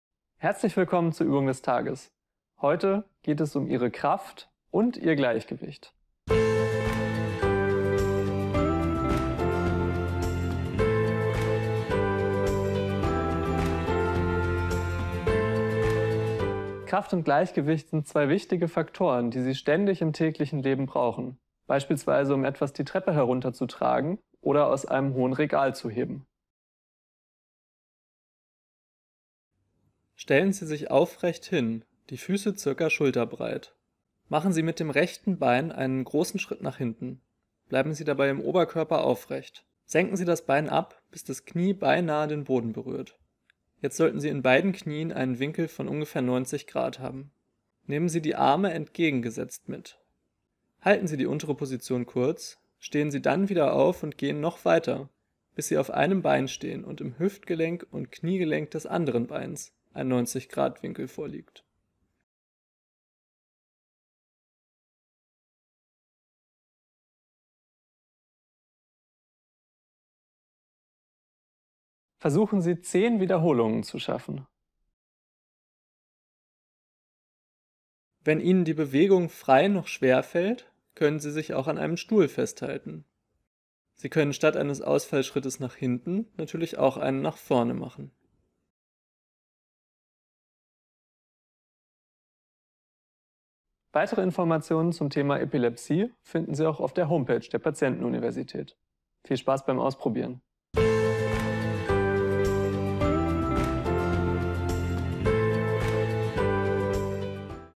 Ausfallschritt
Der Ausfallschritt - kräftigen Sie Ihre Beinmuskulatur und gönnen Sie sich eine kurze Trainingseinheit!